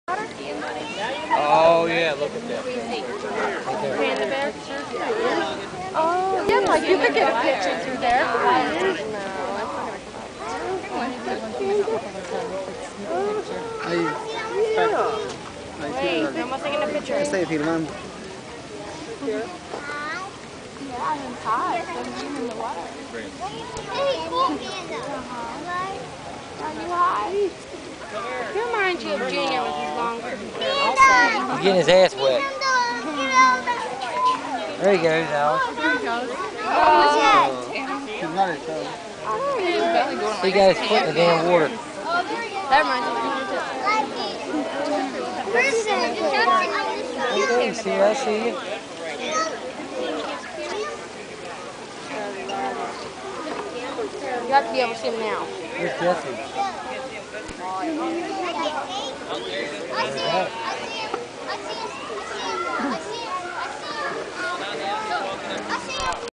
PANDA - Washington D.C Zoo
la vida del panda en un habita del zoologico de d.c que lo disfruten..!!